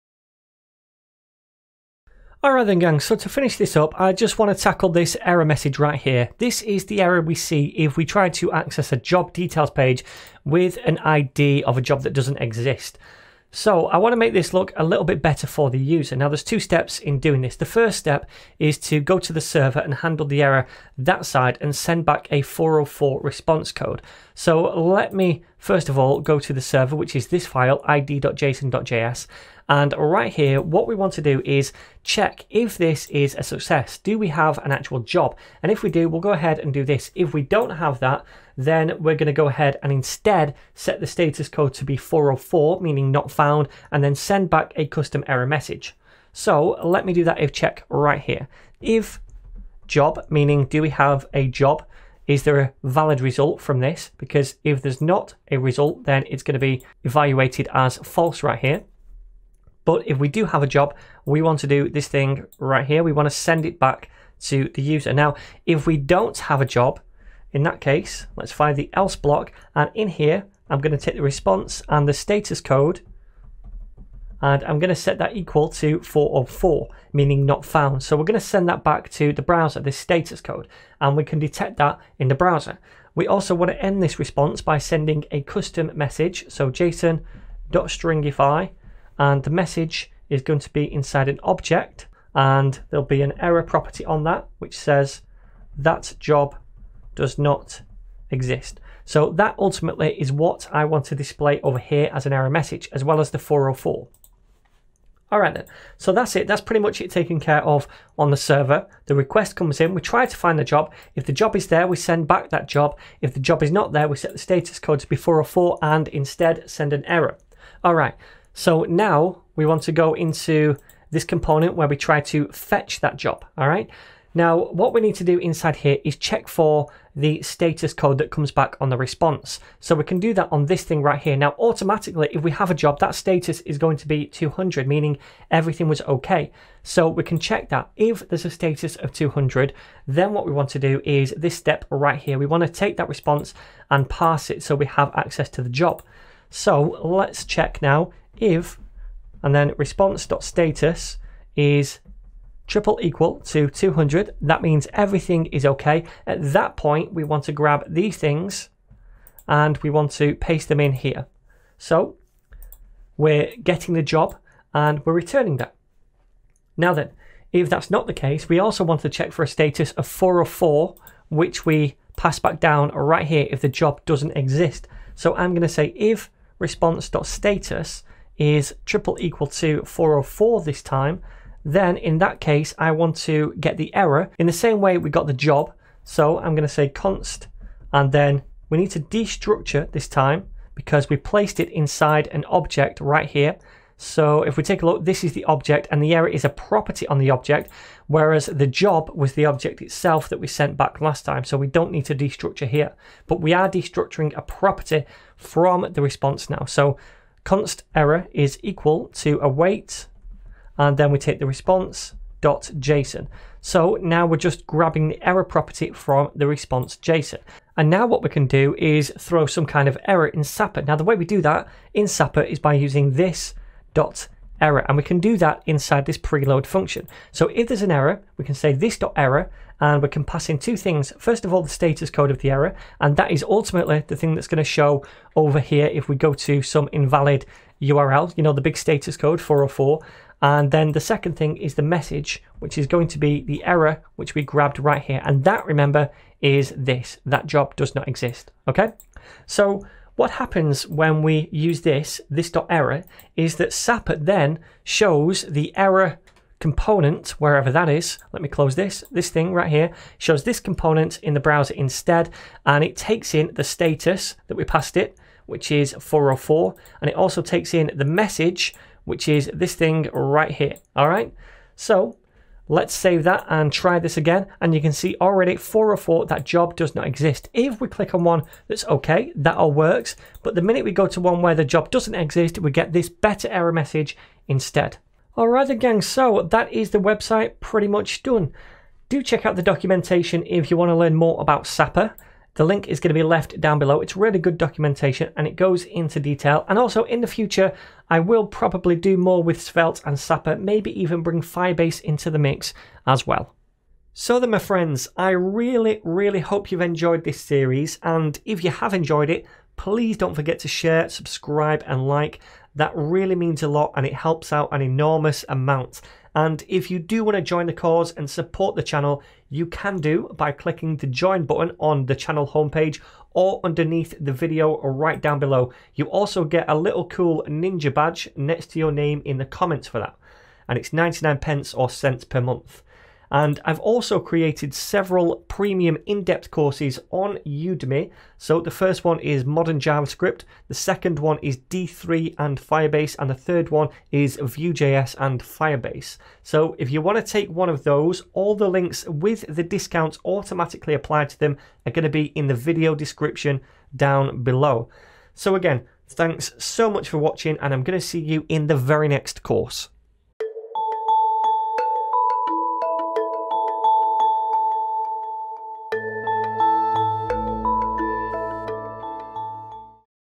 Sapper Tutorial #12 - Error Messages
Hey gang, in this Final Sapper tutorial we'll create some custom errors to show the user when things go wrong!